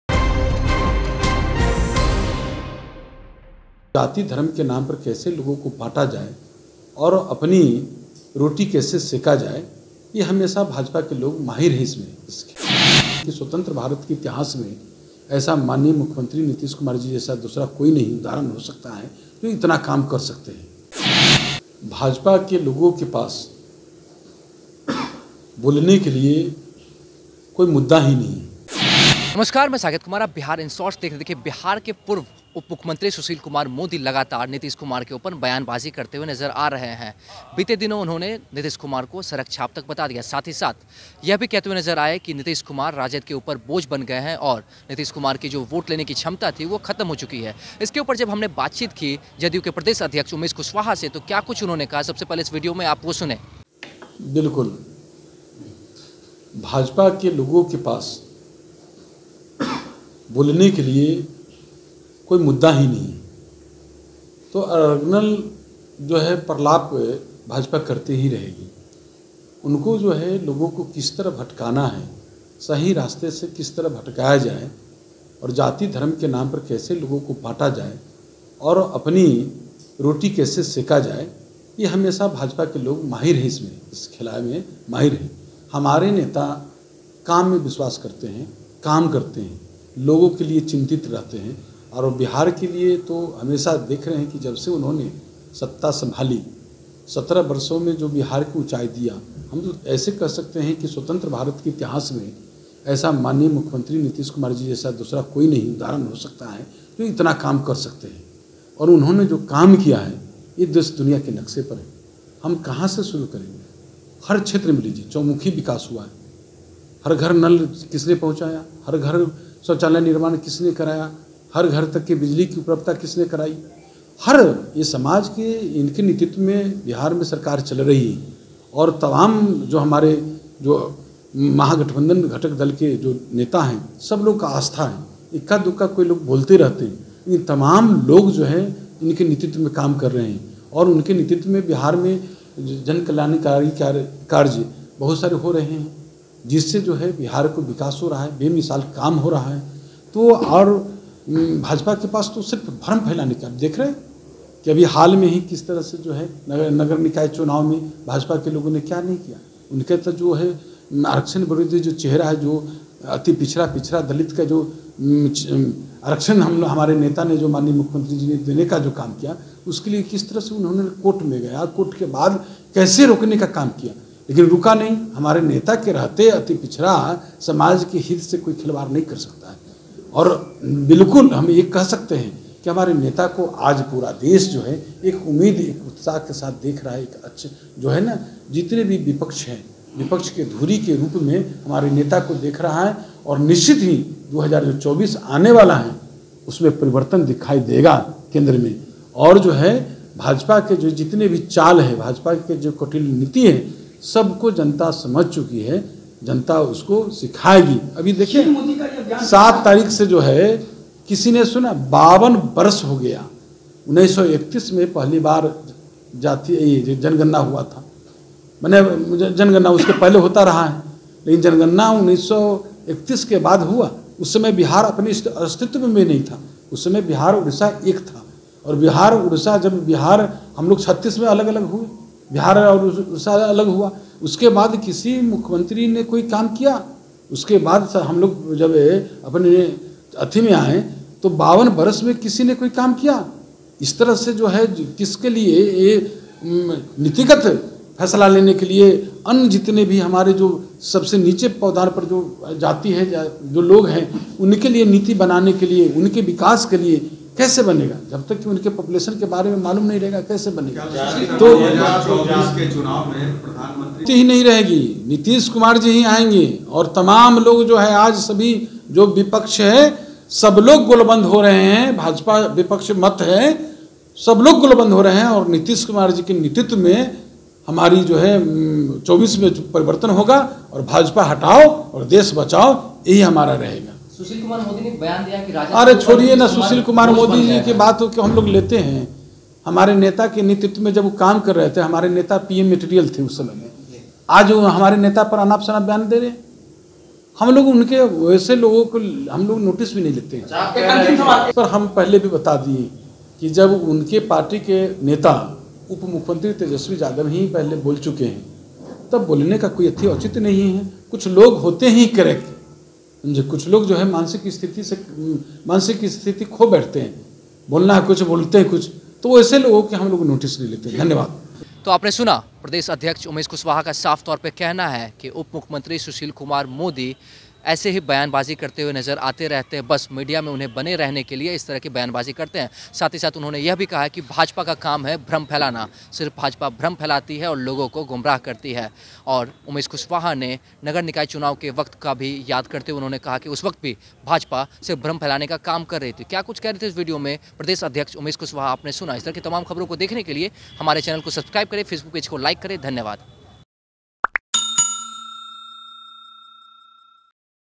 JDU के Umesh Kushwaha बोले- Sushil Modi की बातों को नोटिस भी नहीं लेते, 2024 में BJP का सफाया तय
नीतीश की पार्टी जदयू ने कह दिया, 2024 में बीजेपी का सफाया तय, सुशील मोदी की बातों को नोटिस भी नहीं लेते..देखें वीडियो...